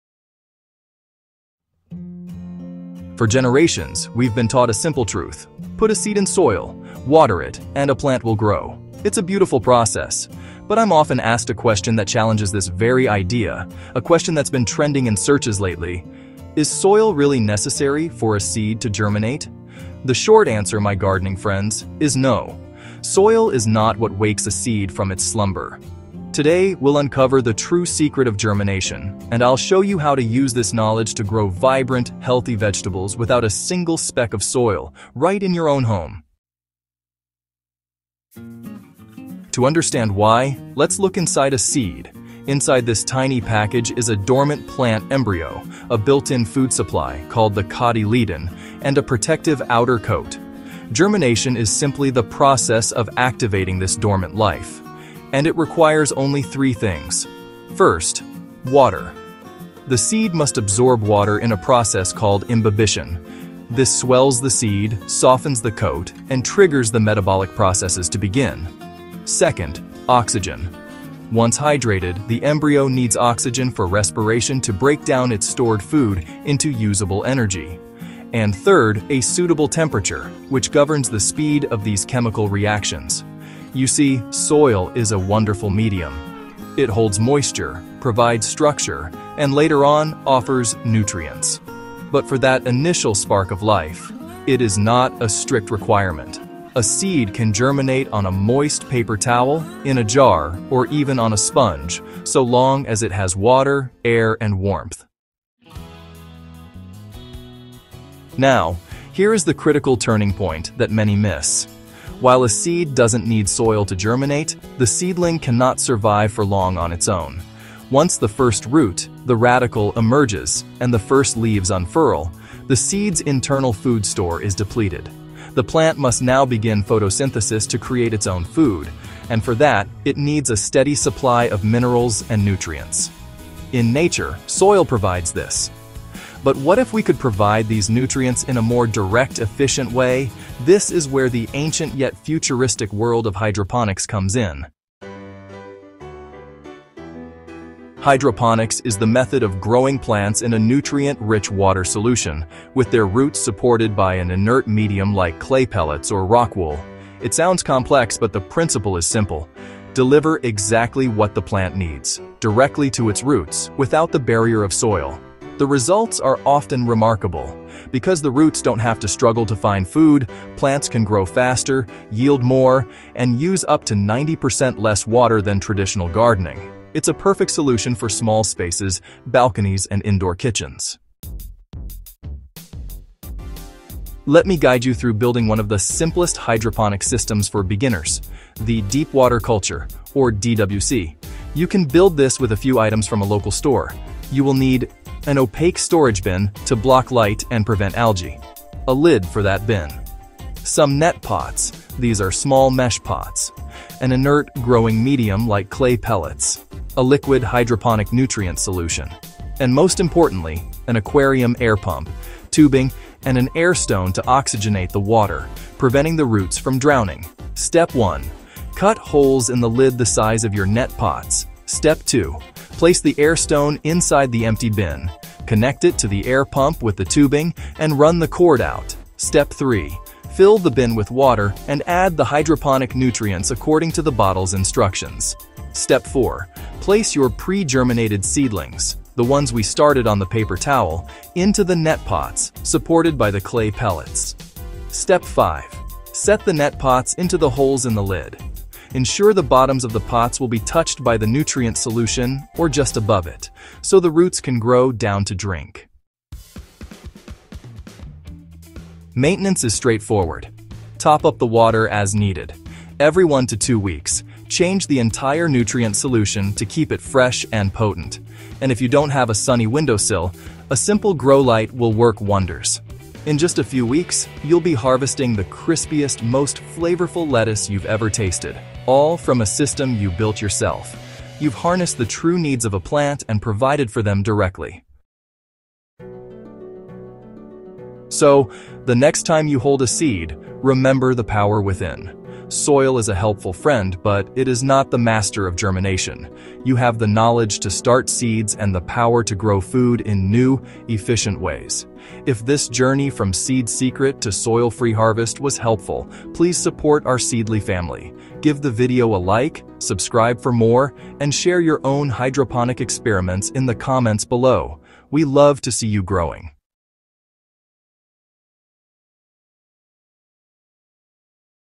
Is The Soil Necessary For The Seed To Germinate? Start Seed Indoors. Grow Hydroponic Vegetables
Is soil necessary for seeds to germinate?The answer will surprise you! This video dives deep into the science of seed germination and reveals how you can use this knowledge to grow your own food without any soil at all.

We start by explaining what seeds truly need to sprout (it's not soil!) and then guide you, step-by-step, through building a simple, inexpensive Deep Water Culture (DWC) hydroponic system at home. Perfect for growing lettuce, herbs, and vegetables in small spaces like apartments, balconies, and kitchens.

In this video, you'll learn:
➤The 3 actual requirements for seed germination (Hint: soil isn't one of them).
➤The scientific process of "imbibition" and how it wakes up a seed.
➤A full tutorial on building a beginner-friendly hydroponic system with a storage bin.
➤How to mix nutrients and maintain your soil-free garden for maximum growth.
➤Why hydroponics can be faster and use less water than traditional gardening.

Start your soil-free gardening journey today!